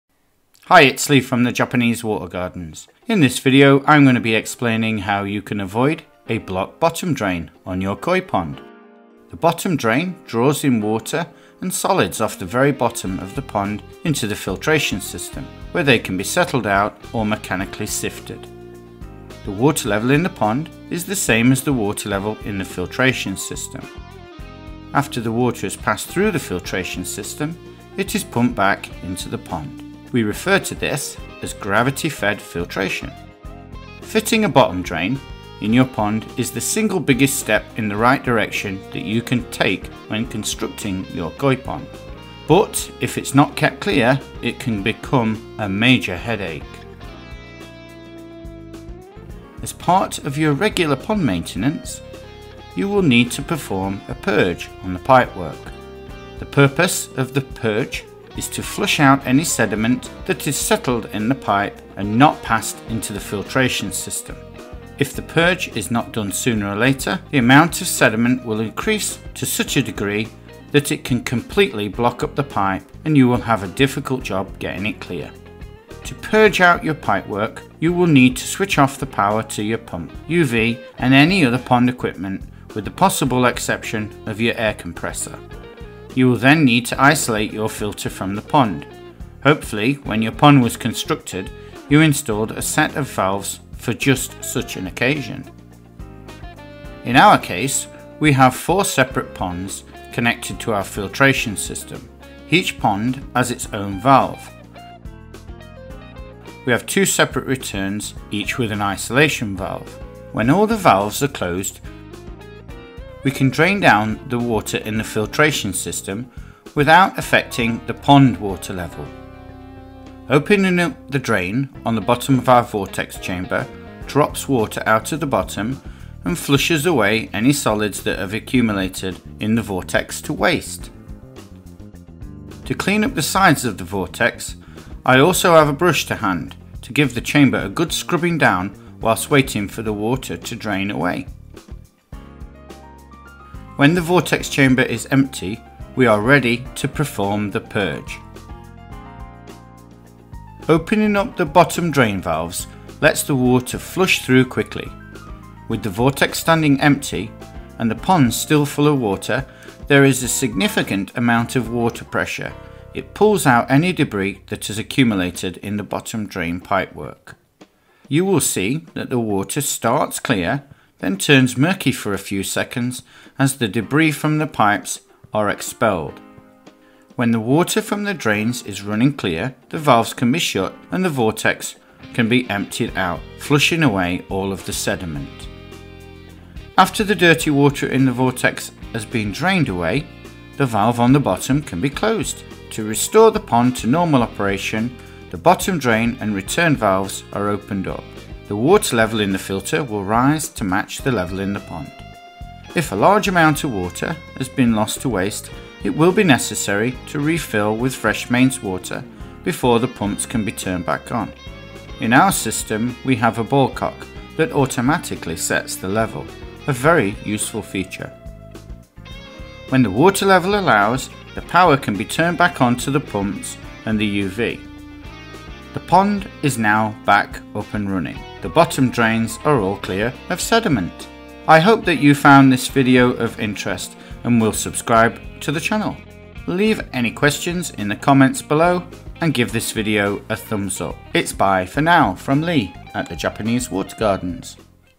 Purge out your bottom drain!
Hi its Lee from the Japanese water gardens, in this video I am going to be explaining how you can avoid a blocked bottom drain on your Koi pond.

The bottom drain draws in water and solids off the very bottom of the pond into the filtration system were they can be settled out or mechanically sifted.

The water level in the pond is the same as the water level in the filtration system.

After the water has passed through the filtration system it is pumped back to the pond.

We refer to this as gravity fed filtration.

Fitting a bottom drain in your pond is the single biggest step in the right direction you can take when constructing your Koi pond.

But if it's not kept clear it can become a major headache.

As part of your regular pond maintenance you will need to perform a purge on the pipework.

The purpose of the purge is to flush out any sediment that has settled in the pipe and not passed into filtration system. If the purge is not done sooner or later the amount of sediment will increase to such a degree that it will completely block up the pipe and you will have a difficult job getting it clear.

To purge your pipe work you will need to switch of power to your pump, UV and any other pond equipment with the possible exception of your air compressor.

You will then need to isolate you filter from the pond. Hopefully when your pond was constructed you installed a set of valves for just such an occasion.

In our case we have a four ponds connected to our filtration system, each pond has its own valve. We have two separate returns each with an isolation valve.

When all the valves are closed we can drain down the water in the filtration system without affecting the pond water level.

Opening up the drain on our vortex chamber drops the water out of the bottom and flushes any solids that have accumulated in the vortex to waste.

To clean up the sides of the vortex I also have a brush to hand to give the chamber a good scrubbing down whilst waiting for the water to drain away.

When the vortex chamber is empty we are ready to perform the purge.

Opening  bottom drain valves let's the water flush through quickly. With the vortex standing empty and the ponds still full of water there is a significant amount of water pressure it pulls out any debris that has accumulated in the bottom drain pipework.

You will see the water starts clear then turns murky for a few seconds as the debri from the pipes is expelled.

When the water from the drains is running clear the valves can be shut and the vortex will empty out again and flush away all the sediment.

After the dirty water in the vortex has drained away the valve on the bottom can be closed.

To restore the pond to normal operation the bottom drain and return valves are opened up. The water level in the filter will rise to match the level in the pond.

If a large amount of water has been lost to waste it will be necessary to refill with fresh mains water before the pumps can be turned back on.

In our system a ballcock automatically sets the level.

When the water level allows the power can be turned back on to the pumps and UVs. The filter and pond is back up and running.

The bottom drains are all clear of sediment!

Its bye for now from Lee at the Japanese Water Gardens.